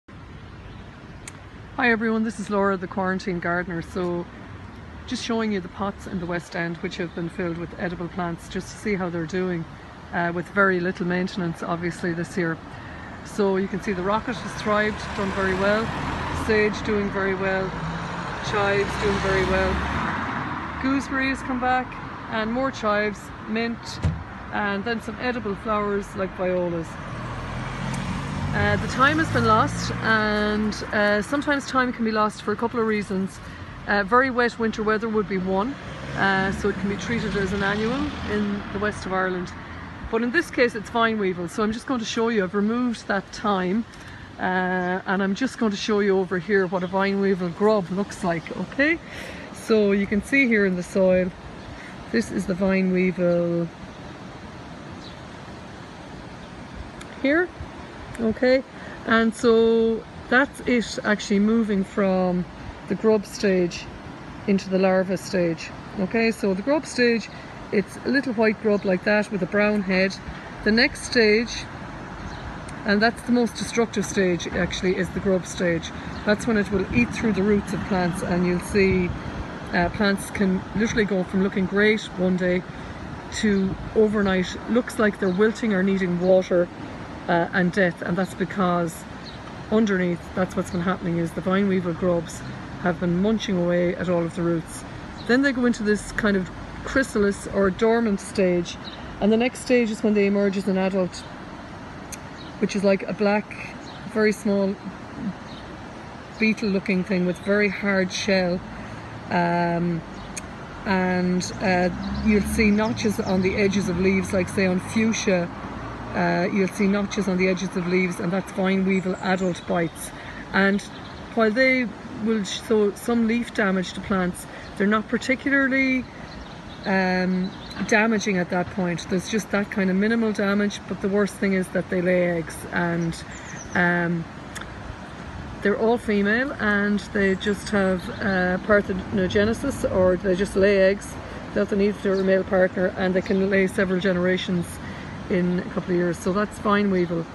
Vine Weavel in the Galways Westend edible pots with The Quarantine Gardener
Galway's Westend edible plant pots are thriving, well mostly as the Quarantine Gardener, Laura O'Connor eplains in this vlog.
The rocket sage chives gooseberries mint violas are all ready to eat so if you are down in the Westend galway help yourself
If you are a visitor then please feel free to copy and share this initiative in your own community.
We are all so grateful for the abundance of food we harvest from the pots every year.
There are 3 within the area of Dr Clare Apothecary for starters!
Even Kai Restaurant have one just outside the door too.
So help yourselves and pay it forward.